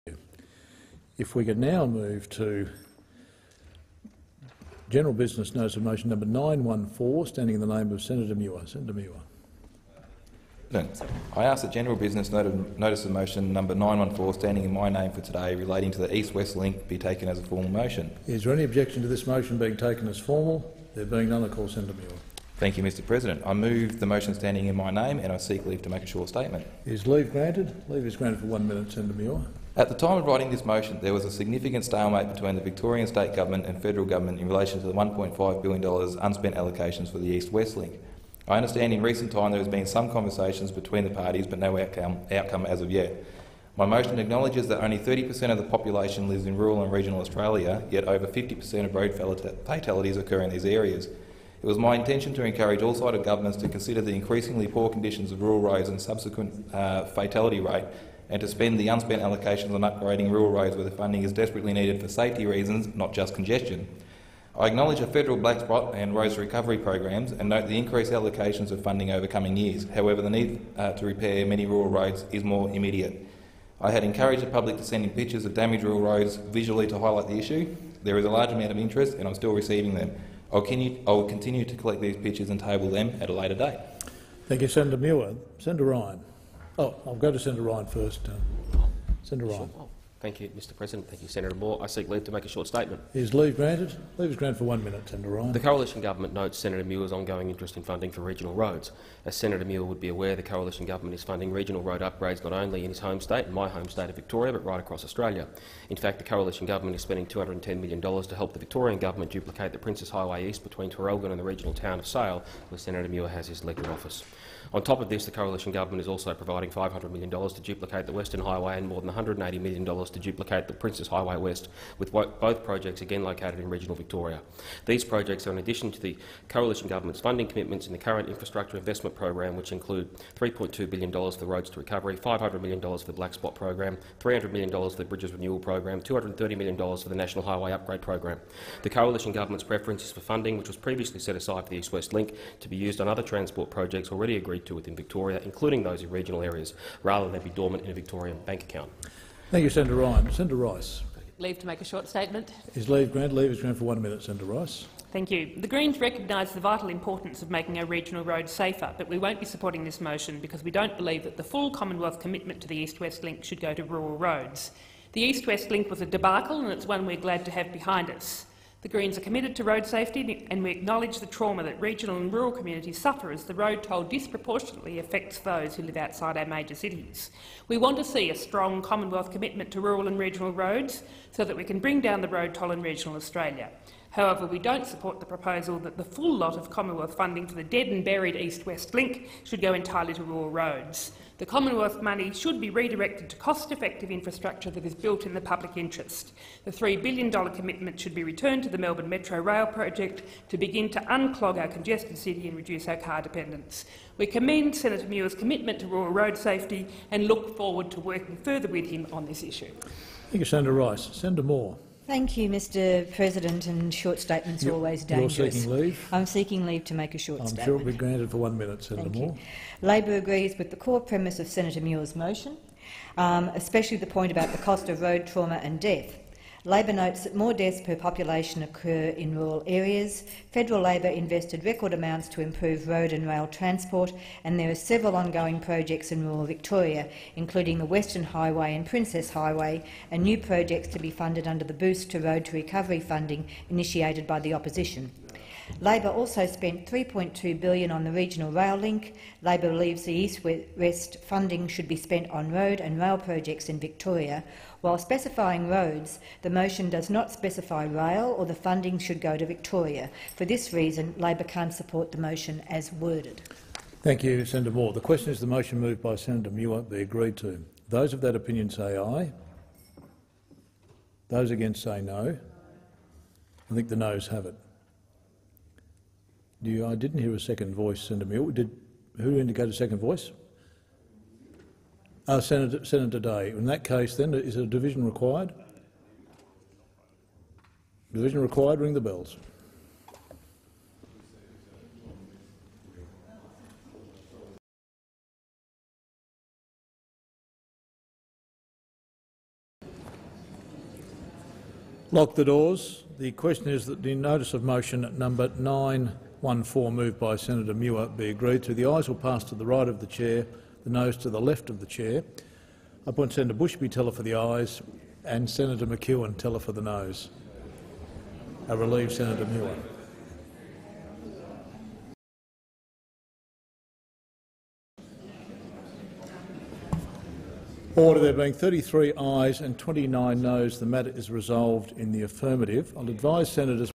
Senator Ricky Muir | Rural Road Funding Motion | 25-Nov-2015
At the time of writing this motion there was a significant stalemate between the Victorian state government and the federal government in relation to the $1.5 billion unspent allocations for the East West Link.

I understand that in recent time there has been some conversations between the parties but no outcome as of yet.

My motion acknowledges that only 30 per cent of the population lives in rural and regional Australia yet over 50 per cent of road fatalities occur in these areas.

It was my intention to encourage all sides of government to consider the increasingly poor conditions of rural roads and the subsequent fatality rate and to spend the unspent allocations on upgrading rural roads, where the funding is desperately needed for safety reasons, not just congestion.

I acknowledge the federal Black Spot Program and Roads to Recovery program and note the increased allocations of funding over coming years. However, the need to repair many rural roads is more immediate. I had encouraged the public to send in pictures of damaged rural roads to visually highlight the issue. There is a large amount of interest, and I am still receiving them. I will continue to collect these pictures and will table them at a later date.

Senator Muir calls on the Government to withdraw the allocation of federal funding set aside for East West Link and to reallocate this funding to projects to improve rural road infrastructure.

Details of motion:

I give notice that on the next day of sitting I shall move that:
The Senate—
a) notes that:
   i. There is a political stalemate between the Government and the Victorian state Government in relation to the East West link.
   ii. There is approximately $1.5 billion in unspent allocations to the East West link.
   iii. Evidence presented to the Rural and Regional Affairs and Transport References Committee – Aspects of road safety in Australia by the National Rural Health Alliance, demonstrated that those who live outside the major cities make up 30 per cent of the population and 52 per cent of deaths due to land transport accidents.
   iv. Rural roads play a vital role in putting food on city tables and bringing resources to urban areas.
   v. The annual economic cost of road crashes in Australia is enormous—estimated at $27 billion per annum—and the social impacts are devastating.
   vi. Many rural roads are in dangerously poor condition and in desperate need of repair.
   vii. Some local councils in Victoria do not have the necessary funding to fully maintain the extensive road networks in their electorate due to proposed rate caps and the federal freeze in Financial Assistance Grants.
   viii. A life in rural and regional Australia is just as important as a life in urban Australia.
   ix. Until there is a change of Victorian State Government or a change in Federal Government a resolution to the East West link is unlikely.
   x. There is a strong need in Victoria to invest in rail and road infrastructure in order to ease congestion and improve productivity into the future.

(b) calls on the Government to withdraw the allocation of federal funding set aside for East West Link and to reallocate this funding to projects to improve rural road infrastructure.